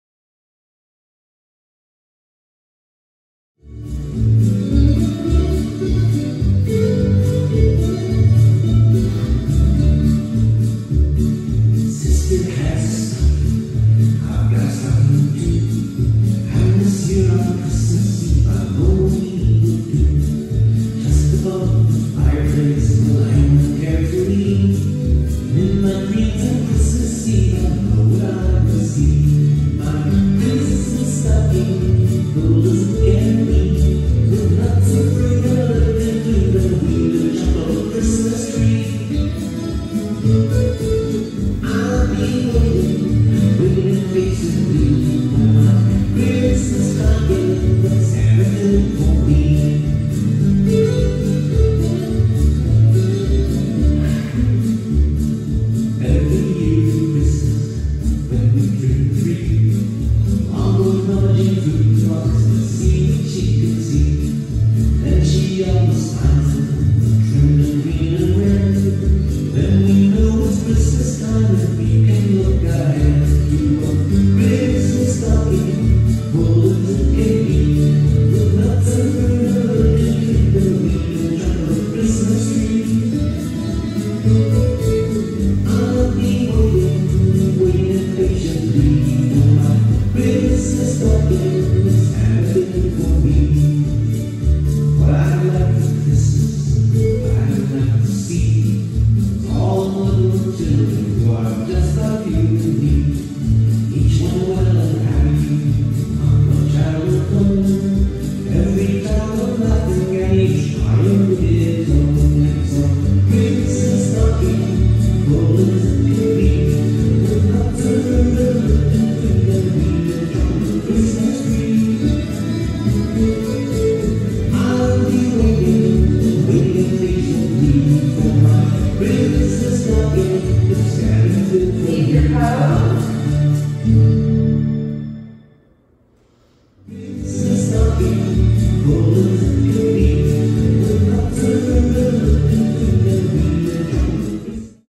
Christmas Stocking
Practice video for our Live Oak Wednesday K-1st grade Ballet class.

Choreography: Shannon McCook//Danceology (Live Oak, FL)